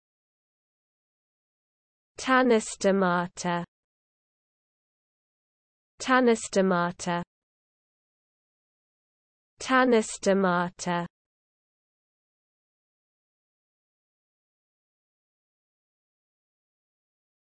How to pronounce Tanystomata (CORRECTLY)
Learn how to correctly pronounce the word Tanystomata  in the US English accent.